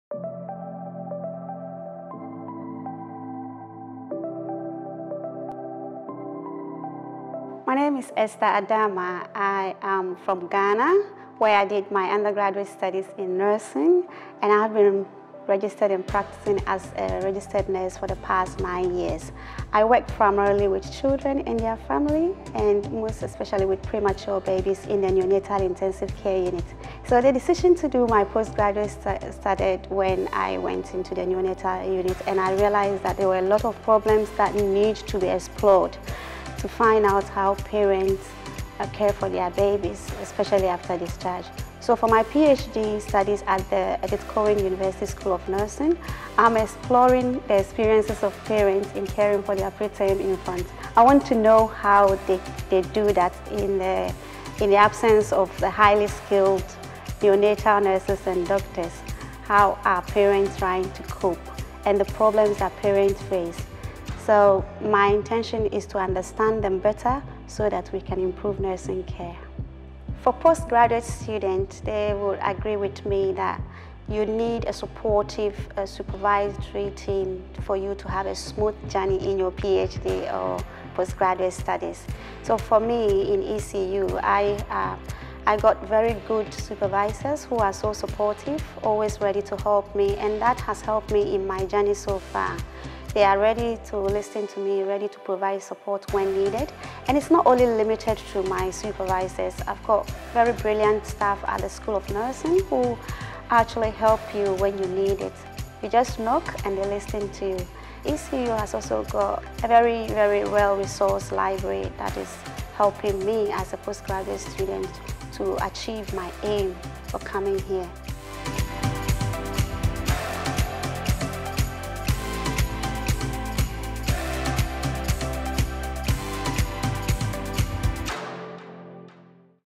Why I'm Studying a PhD With ECU's School of Nursing & Midwifery - Esther's Story
Esther completed her undergraduate Nursing studies in Ghana, and has been a Registered Nurse (RN) for 9 years. She works primarily with children and young families, and premature babies in neonatal intensive care. Esther was inspired to return to study when she started in the neonatal intensive care unit. She's currently completing a Doctor of Philosophy (PhD), exploring the experiences of parents caring for their preterm infants.